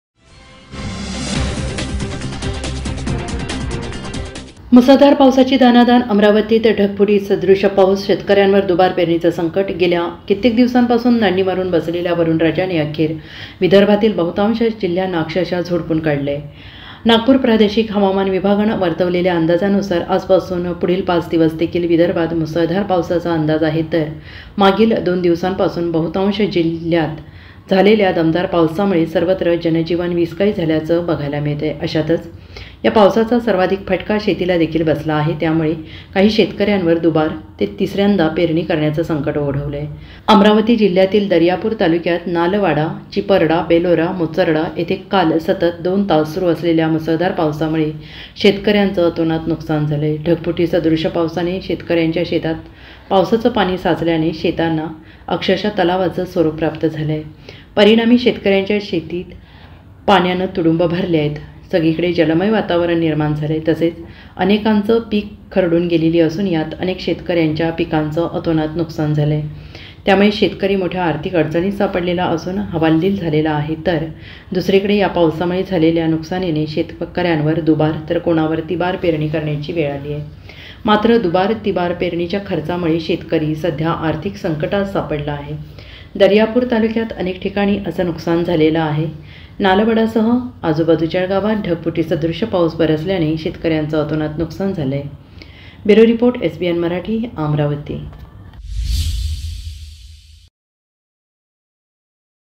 AMRAWATI | मुसळधार पावसाची दाणादाण! अमरावतीत ढगफुटी सदृश्य पाऊस, शेतकर्‍यांवर दुबार पेरणीचे संकट
मुसळधार पावसाची दाणादाण! अमरावतीत ढगफुटी सदृश्य पाऊस, शेतकर्‍यांवर दुबार पेरणीचे संकट